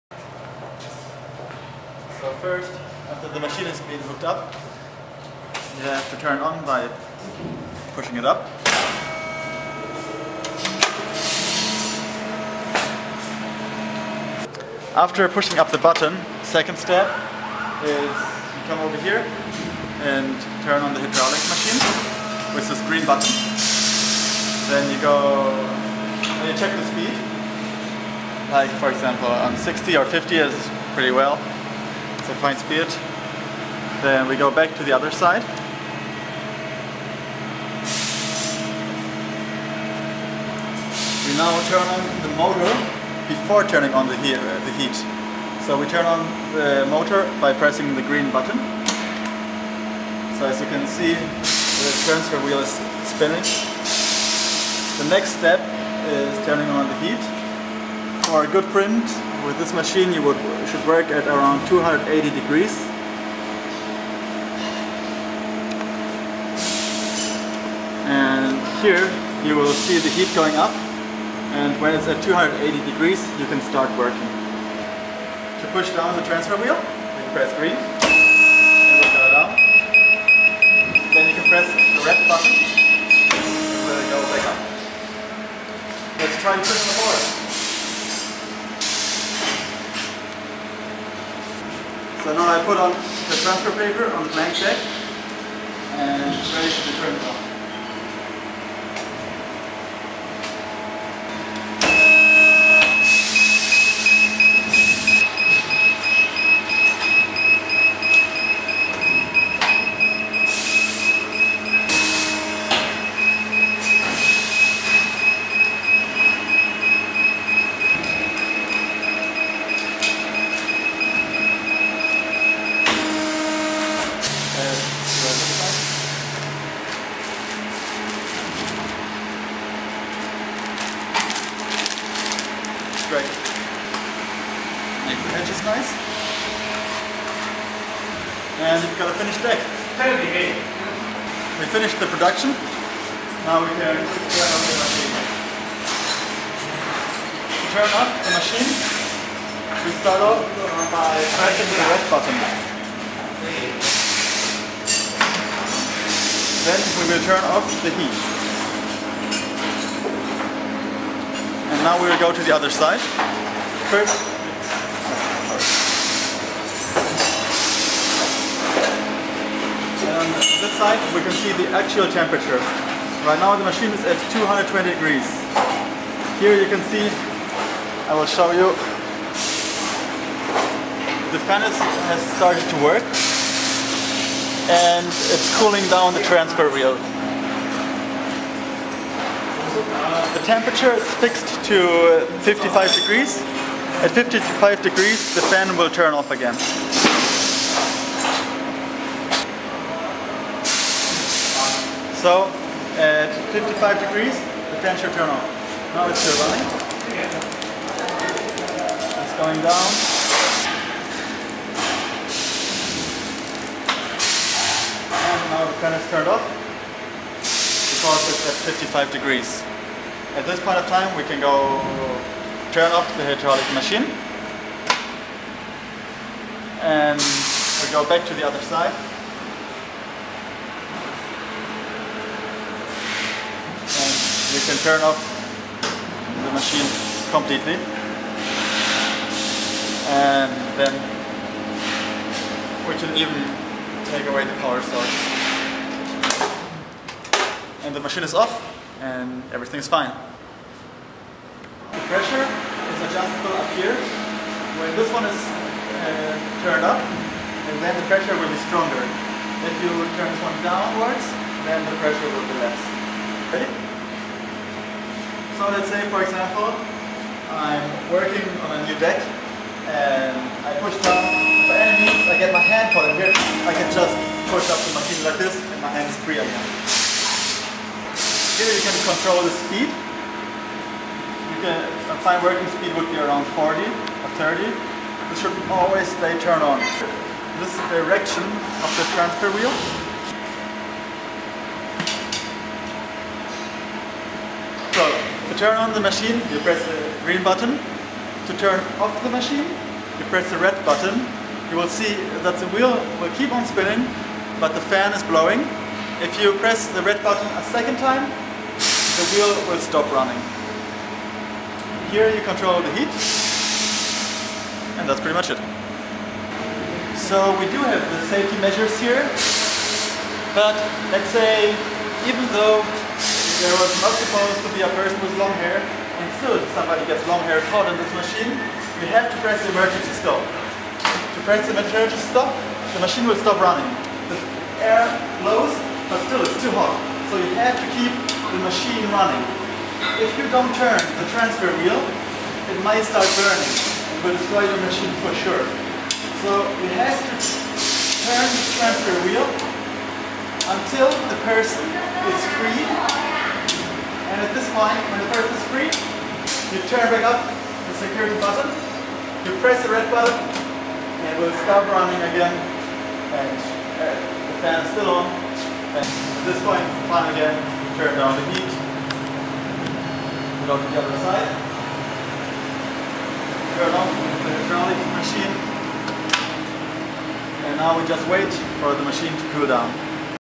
PRECISION Hydraulic Transfer Press
Hydraulic Skateboard Deck Transfer Press to print on skateboard, longboard an cruiser decks.

If you want to see a deck press, check out the video and the webpage: